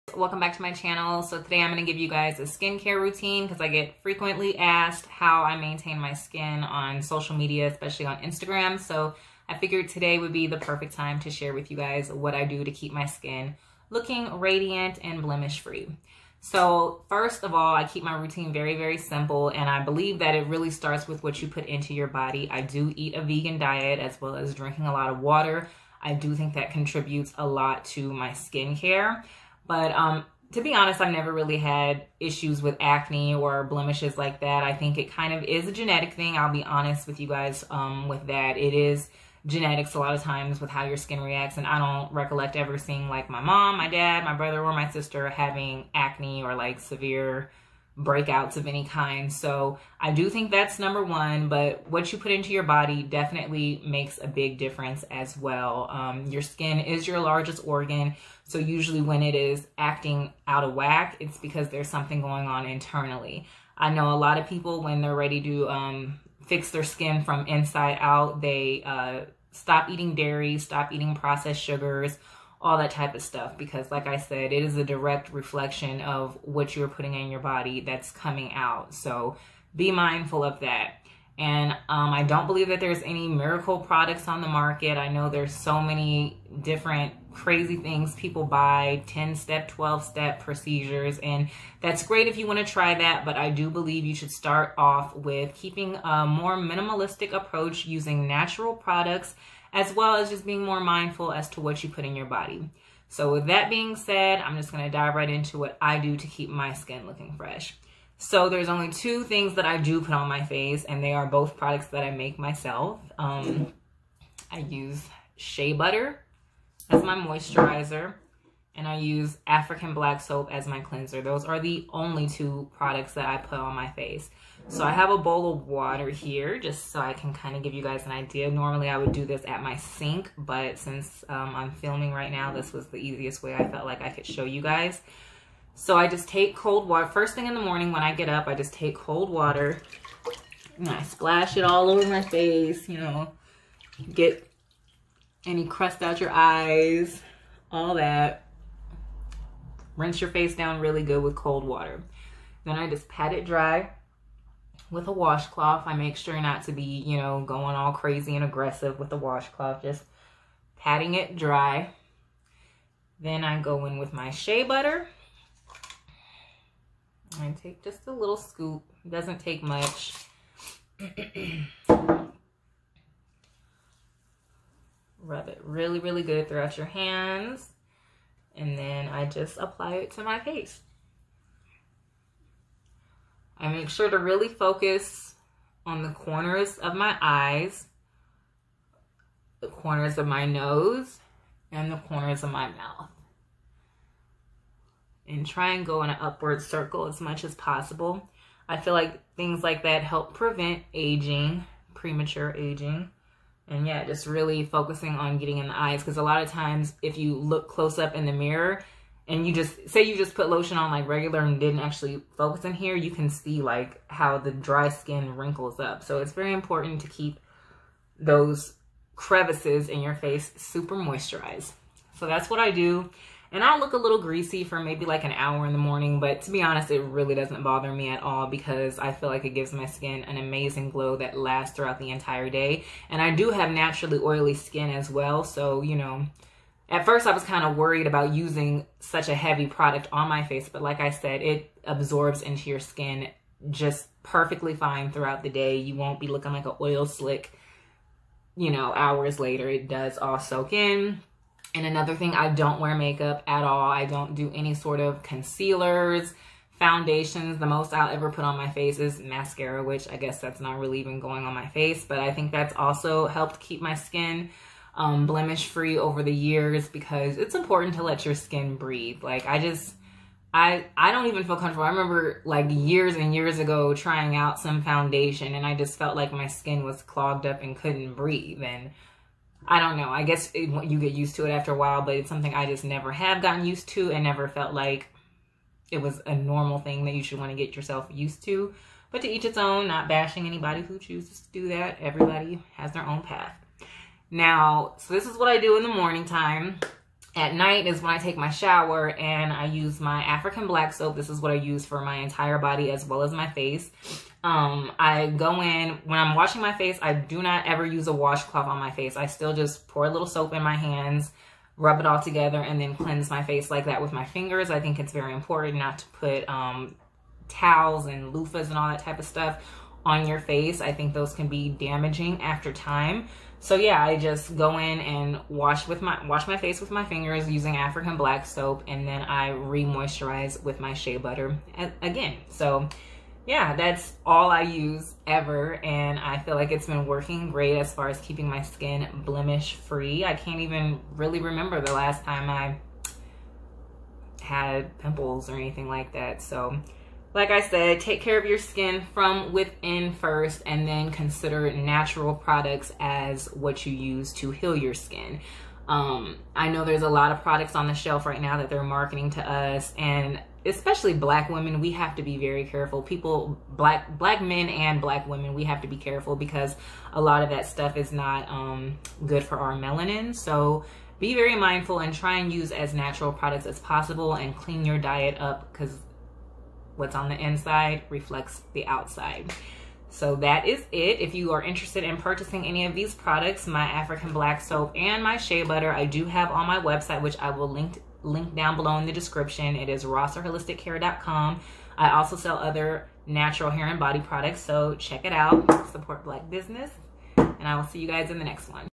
HOW TO MAKE YOUR MELANIN GLOW! SKINCARE ROUTINE!
If you are looking for a simple, affordable skin care routine you need to check this video out!